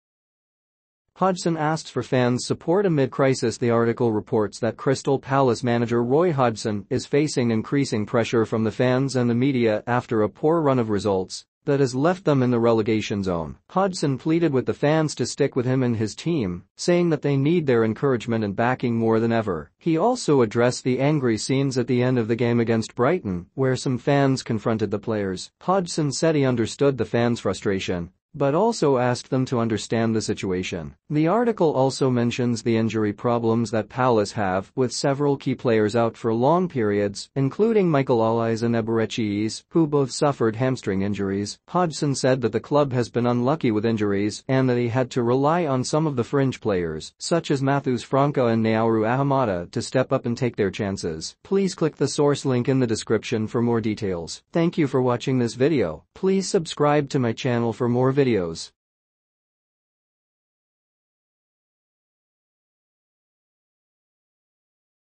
Crystal Palace. Hodgson pleads to Crystal Palace supporters: "we can't do it...
Hodgson asks for fans' support amid crisis
The article reports that Crystal Palace manager Roy Hodgson is facing increasing pressure from the fans and the media after a poor run of results that has left them in the relegation zone. Hodgson pleaded with the fans to stick with him and his team, saying that they need their encouragement and backing more than ever. He also addressed the angry scenes at the end of the game against Brighton, where some fans confronted the players. Hodgson said he understood the fans' frustration, but also asked them to understand the situation. The article also mentions the injury problems that Palace have, with several key players out for long periods, including Michael Olise and Eberechi Eze, who both suffered hamstring injuries. Hodgson said that the club has been unlucky with injuries, and that he had to rely on some of the fringe players, such as Mathues França and Naouirou Ahamada, to step up and take their chances.